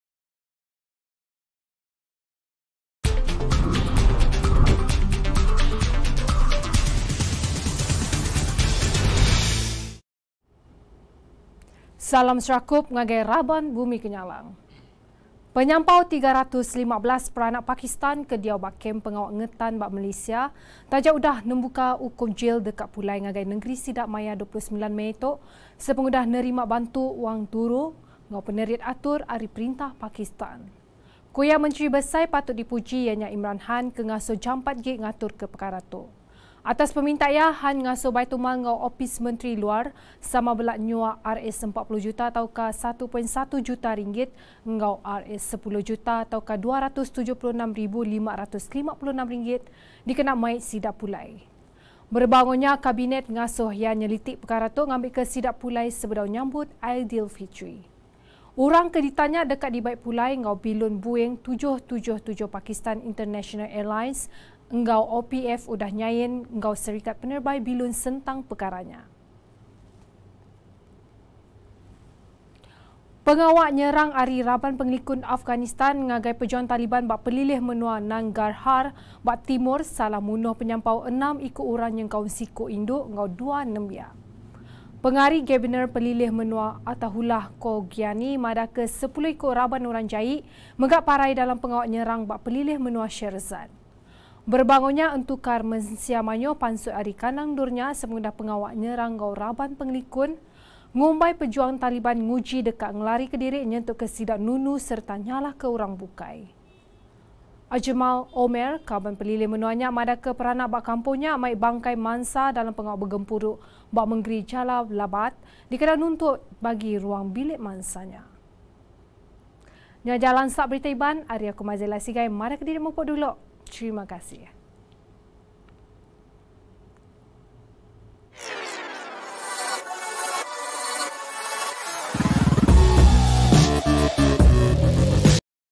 Perintah Pakistan Nanggung Pengawa Nganjung Penyampau 315 Iku Orang Ke Ditan Ba Malaysia
1. PERINTAH PAKISTAN NANGGUNG PENGAWA NGANJUNG PENYAMPAU 315 IKU ORANG KE DITAN BA MALAYSIA

Penyampau 315 peranak Pakistan ke diau ba kem pengawa ngetan ba Malaysia taja udah nembuka ukum jil deka pulai ngagai nengeri sida maya 29 Mei tu, sepengudah nerima bantu wang turu enggau penerit atur ari perintah Pakistan.

Ku iya, Menteri Besai patut dipuji ianya Imran Khan, ke ngasuh jampat gik ngaturka pekara tu.

Atas peminta iya, Khan, ngasuh Baitulmal enggau Opis Menteri Luar, sama bela nyuak Rs40 juta (RM1.1 juta) enggau Rs10 juta (RM276,556) dikena mai sida pulai.

Berebak enggau nya, Kabinet ngasuh iya nyeliti pekara tu ngambika sida pulai sebedau nyambut Aidilfitri.

Orang ke ditan nya, deka dibai pulai enggau bilun buing 777 Pakistan International Airlines eggau OPF udah nyain enggau serikat penerebai bilun sentang pekara nya.

2. RABAN PENGELIKUN AFGHANISTAN SALAH MUNUH ENAM IKU ORANG

Pengawa nyerang ari raban pengelikun Afghanistan ngagai pejuang Taliban ba pelilih menua Nangarhar ba timur salah munuh penyampau enam iku orang nyengkaum siku indu enggau dua nembiak.

Pengari Gabenor pelilih menua Attahullah Khogyani madahka, 10 iku raban orang jai mega parai dalam pengawa nyerang ba pelilih menua Sherzad.

Berebak enggau nya, entukar mensia mayuh pansut ari kandang endur nya sepengudah pengawa nyerang enggau raban pengelikun ngumbai pejuang Taliban nguji deka ngelarika diri nyentukka sida nunu sereta nyalahka orang bukai.

Ajmal Omer, kaban pelilih menua nya, madahka peranak ba kampung nya mai bangkai mansa dalam pengawa begempuru ba mengeri Jalalabad dikena nuntut bagi ruang bilik mansa nya.